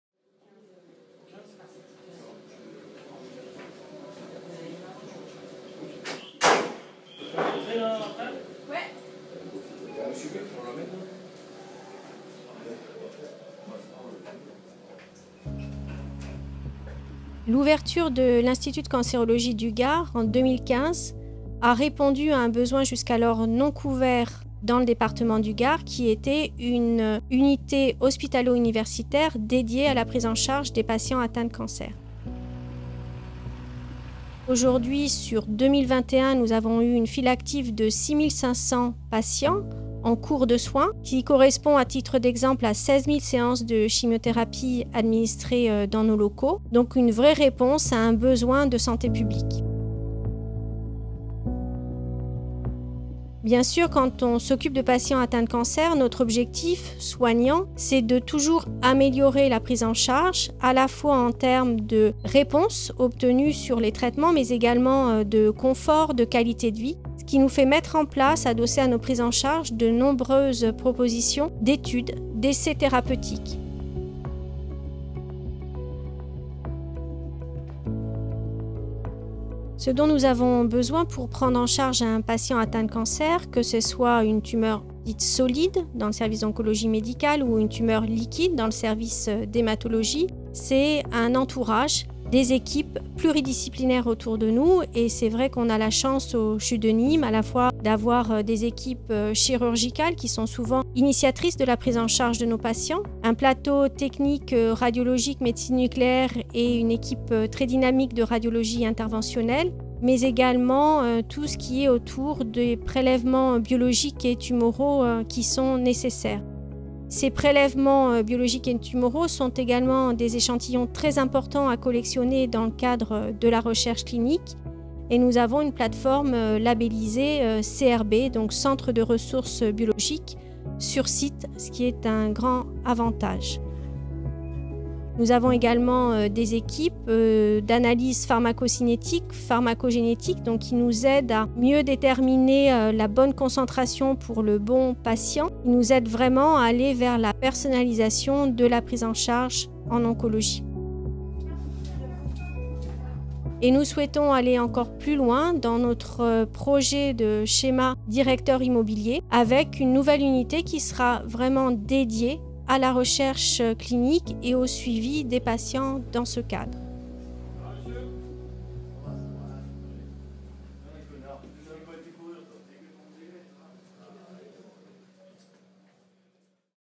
LUNDI CHU - Immersion au service de cancérologie du CHU de Nîmes
Durant tout l’été, Objectif Gard vous propose ses nouvelles rubriques. Tous les lundis, un service du CHU de Nîmes sera mis à l’honneur. Aujourd’hui, cap sur l’ICG, l’institut de cancérologie du Gard. Objectif Gard vous plonge dans le quotidien de ce service et des soignants qui le composent.